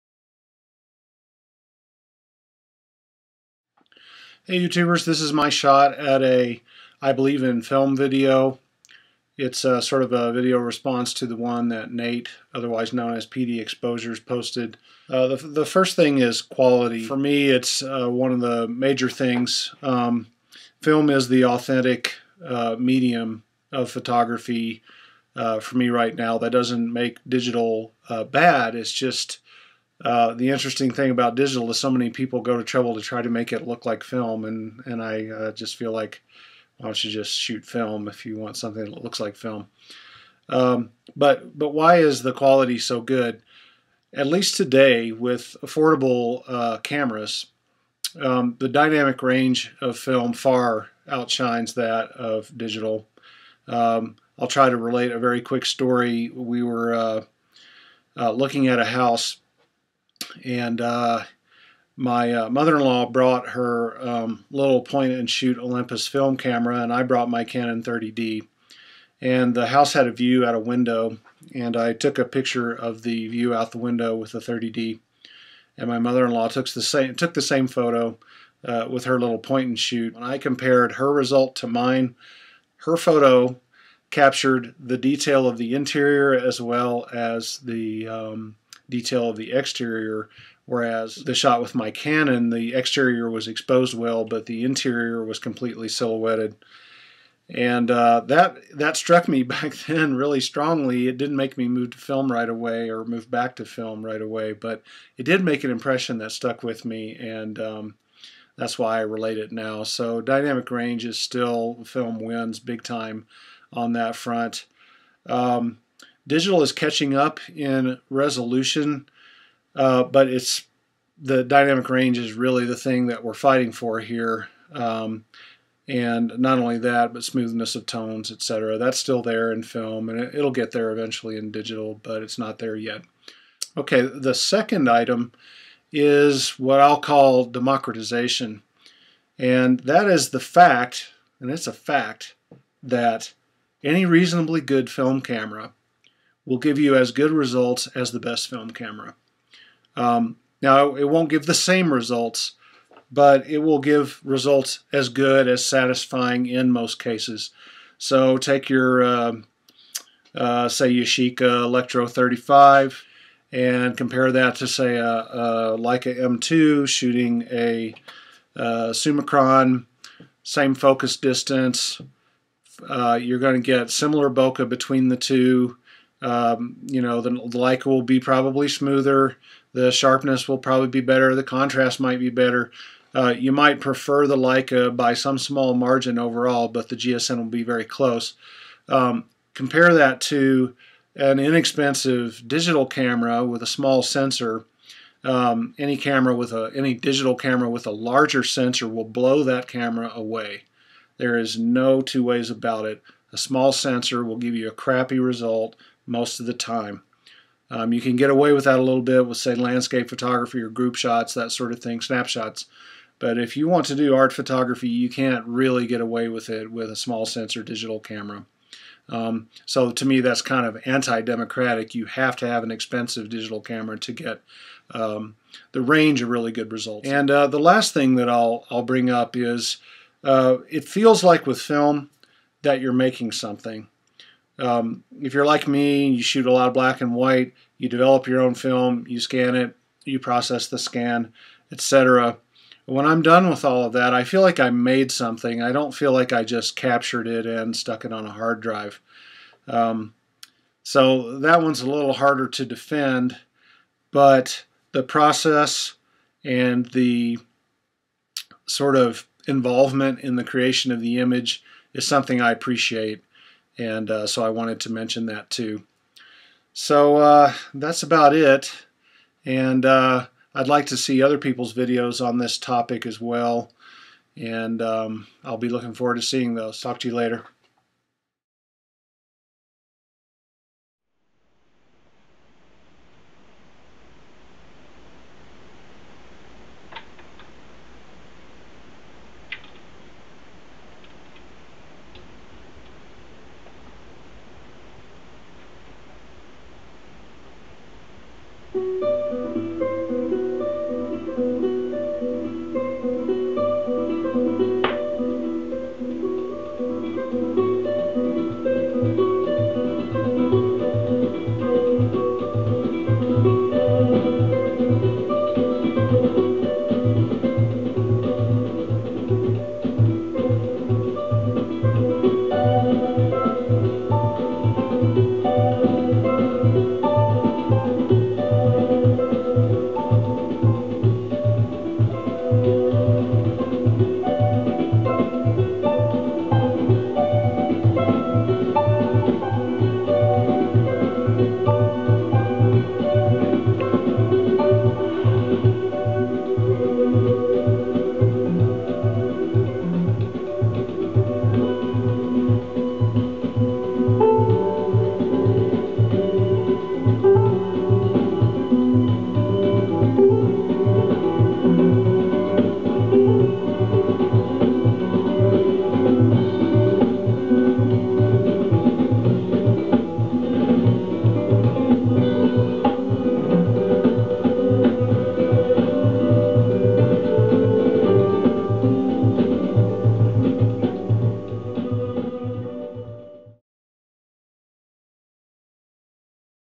Why I
My take on why I believe in film/analog, and somewhat of a video response to pdexposures' (Nate's) post..  Basically my current way of working is:  digital for convenience, film for art.

Since he included a bit of vinyl, I thought I would too.  The music is by Ralph Towner, one of my favorite Jazz artists.